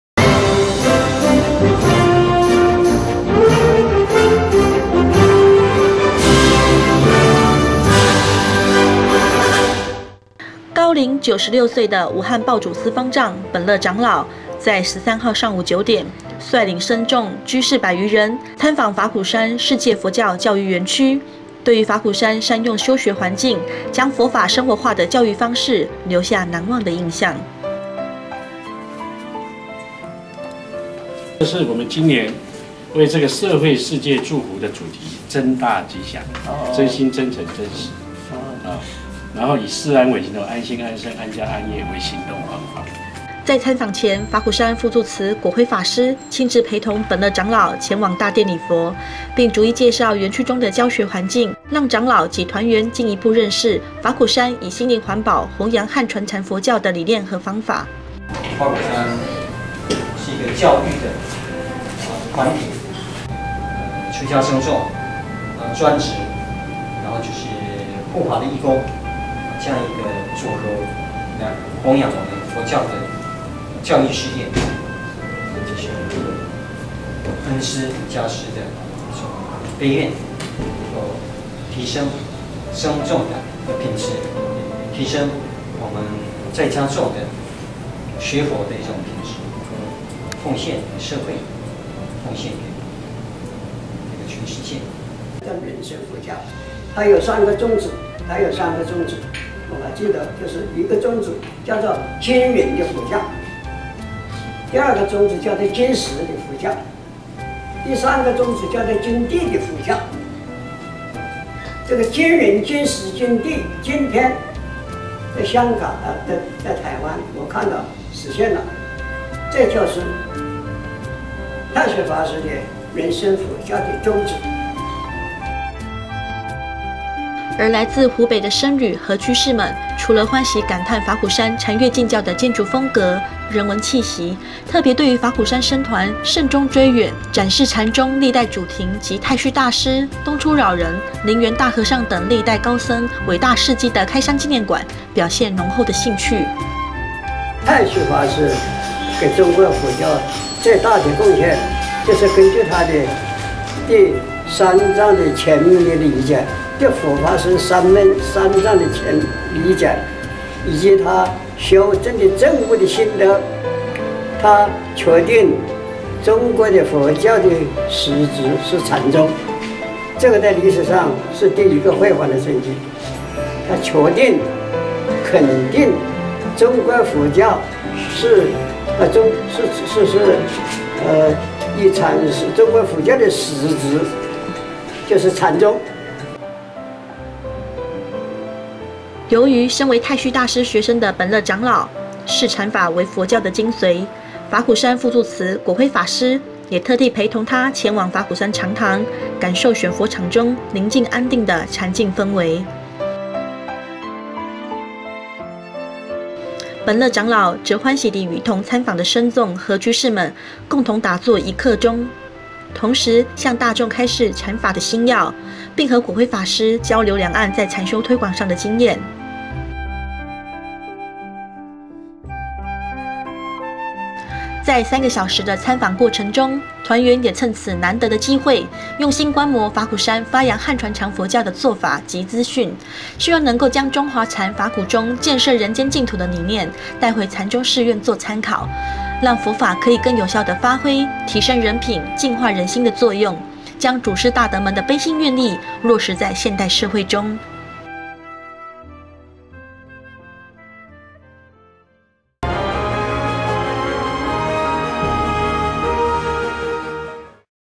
武漢報祖寺本樂長老率團參訪法鼓山
高齡96歲的武漢報祖寺方丈本樂長老，於13日上午九點率領僧眾、居士百餘人，參訪法鼓山世界佛教教育園區。在法鼓山方丈和尚果東法師、副住持果暉法師等僧眾熱情迎接、陪同導覽後，讓長老及團員對於法鼓山以心靈環保弘揚漢傳禪佛教的方法，以及淨化人心，建設淨土的理念，留下難忘的印象。